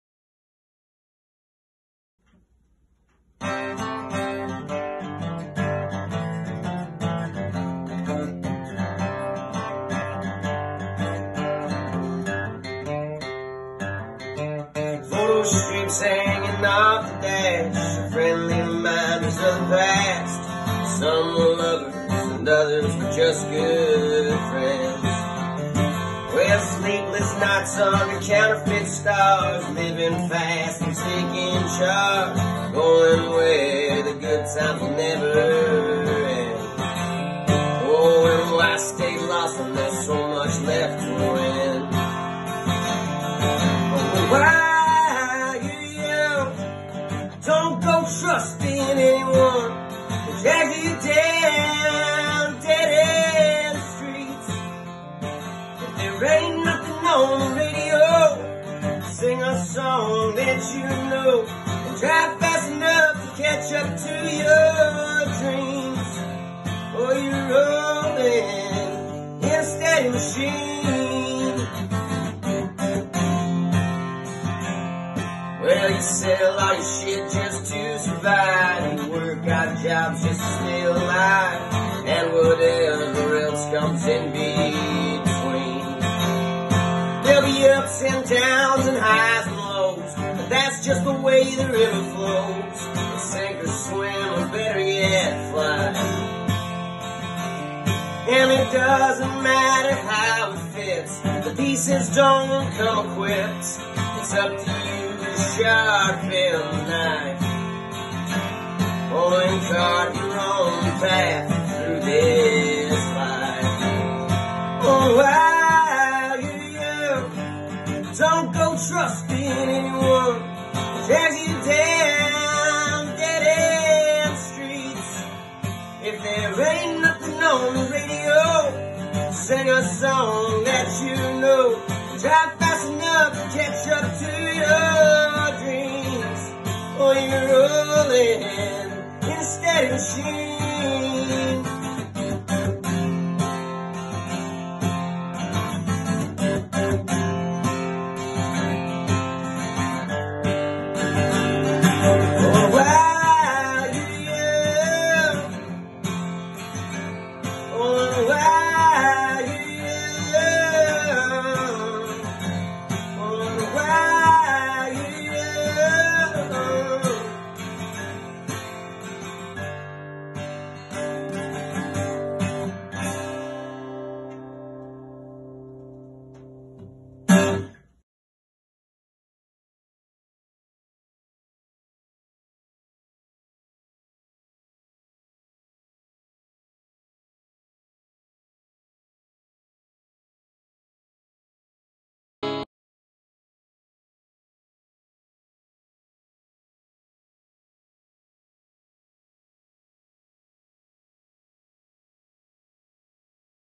Andrew Sheppard Magnolia Roads American Roots Hoedown Virtual Fest 2020.05.17
Andrew Sheppard's set Magnolia Roads American Roots Hoedown Virtual Festival I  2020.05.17.

Inspired by the music that grew from the roots of America at honky-tonks and dive bars, juke joints and roadhouses, backyards and campfires, featuring Magnolia Roads family and friends artists who epitomize American Roots music through their blend of Bluegrass, Blues, Country, Folk, Funk, and Rock n Roll, flavored with the deep grooves of the South and Jam Band psychedelic twirls. It's a family reunion of artists, a love fest and a yang dang doodle good time!